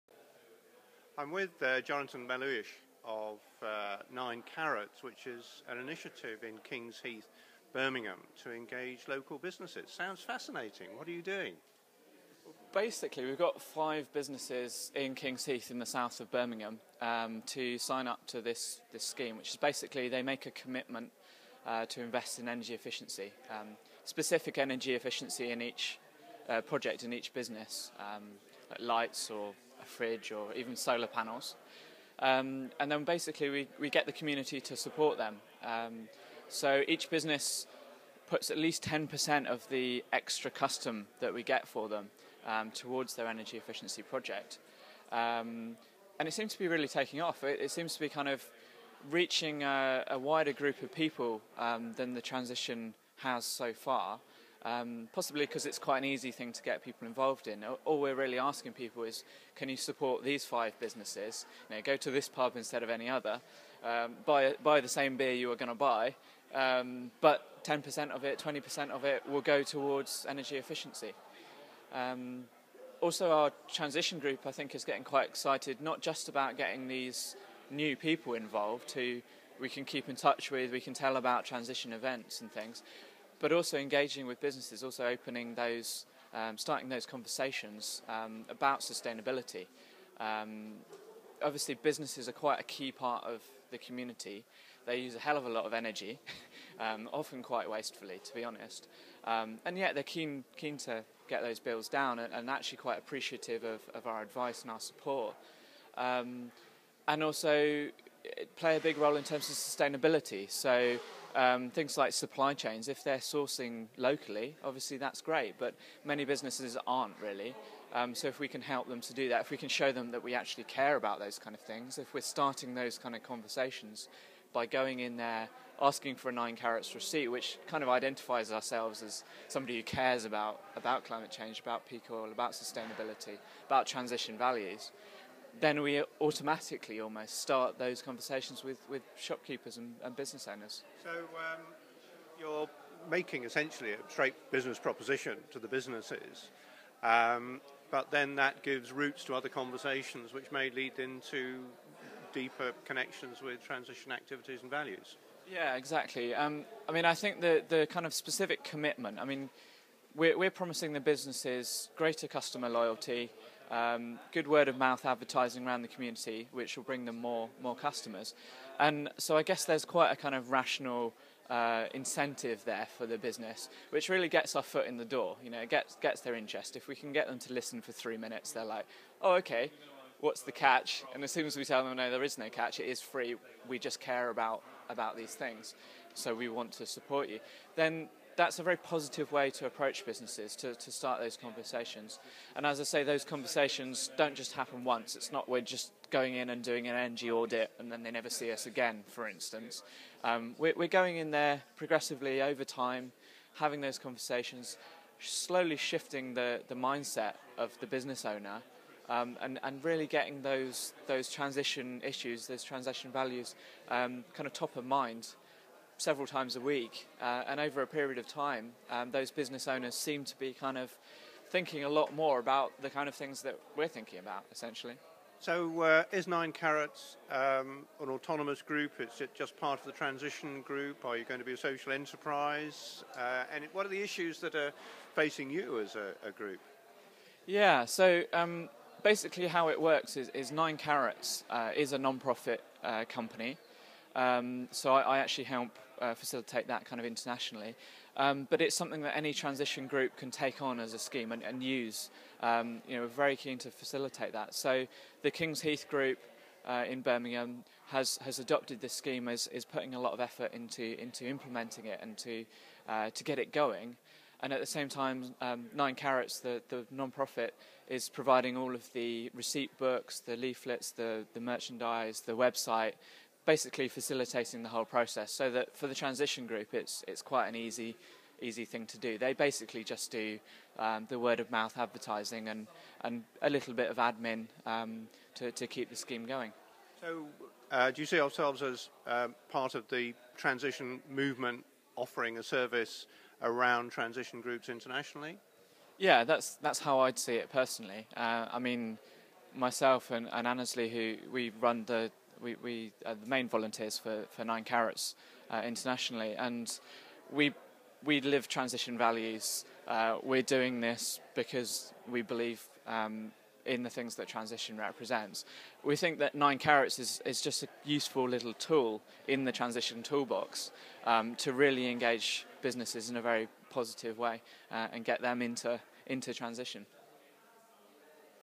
9 carrots engages business in Transition
Jonathan Melhuish explains at Transition 2011 conference how his nonprofit 9 carrots is engaging business in Transition activities.